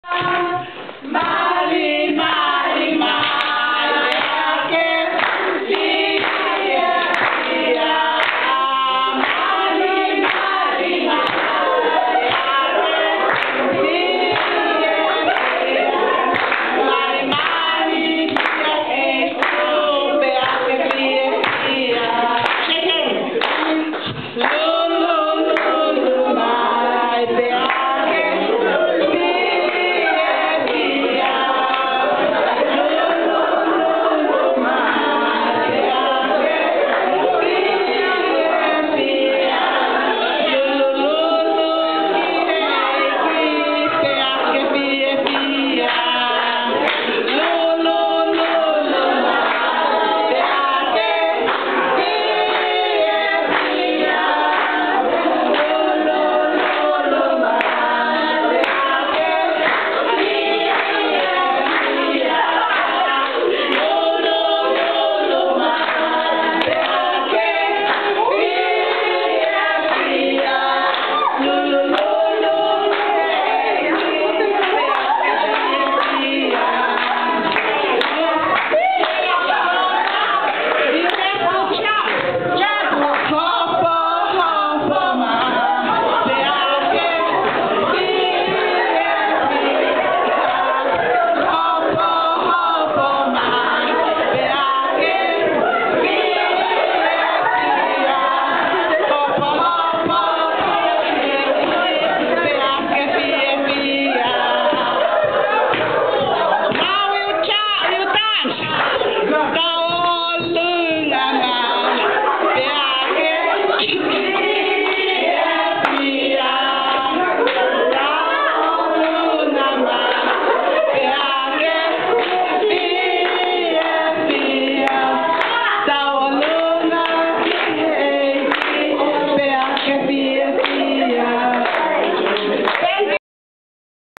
Performance by Girl Guides from Tonga
20 May 2012, 4th Pacific Strategy Meeting, Fiji

Performance by Girl Guides of Tonga during the campfire evening